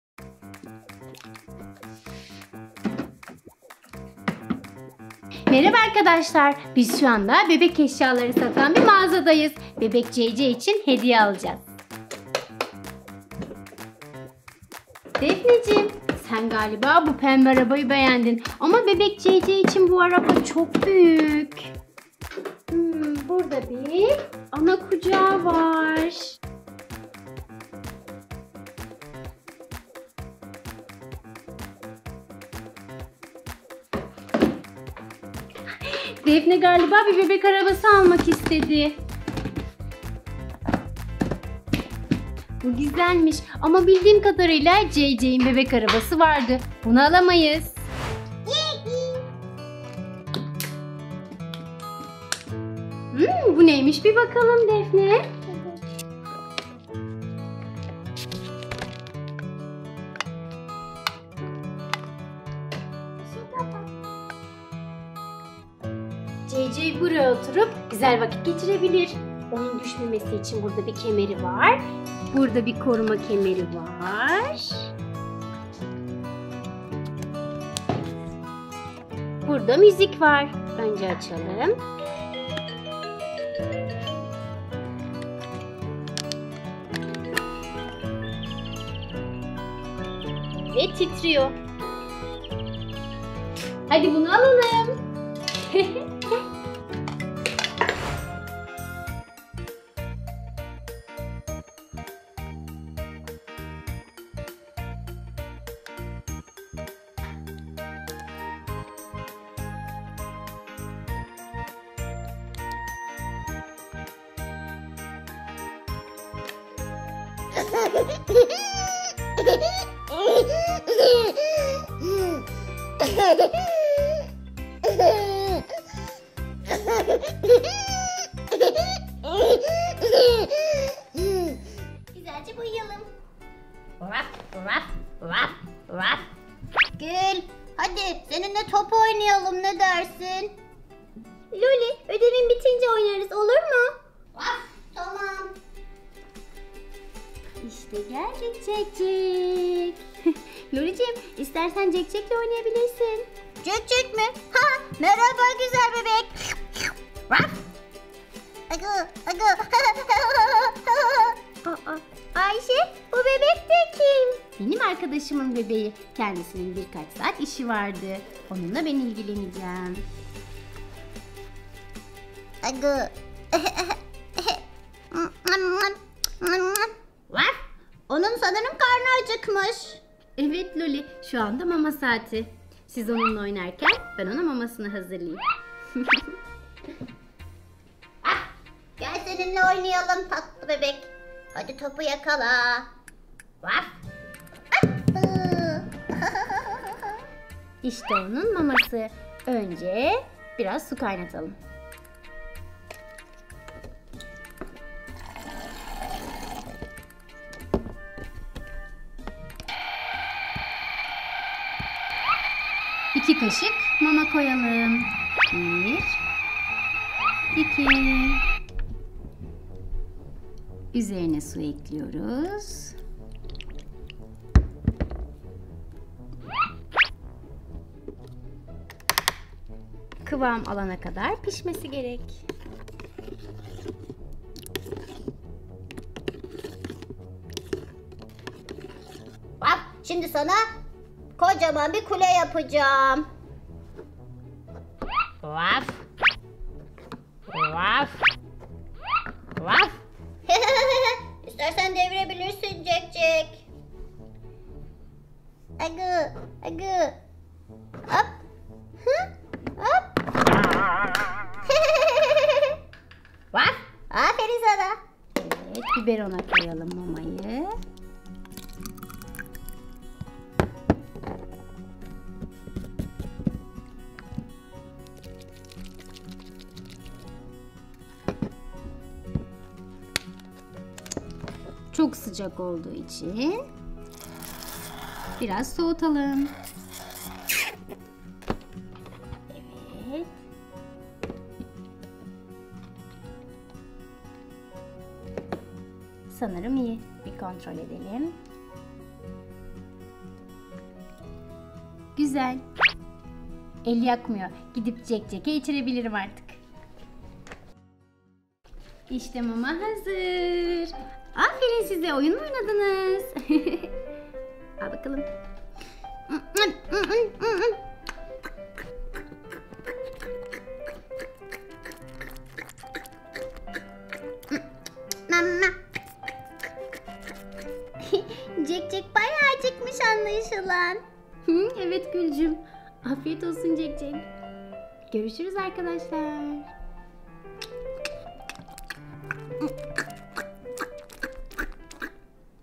JJ baby ile bebek bakma oyunları. Oyuncak bebek için doğum günü hediyesini seçiyoruz Alışveriş oyunu
Ayşe Defne ile bebek mağazasına geldiler. JJ baby ile bebek bakma oyunları oynuyoruz. Bebeğimizin doğum günü olduğu için ona bir hediye alacağız. Alışveriş oyunu başlıyor!

Eğlenceli kız kanalımıza hoşgeldiniz! En sevdiğiniz oyuncaklarınız ile - hep beraber oynayalım.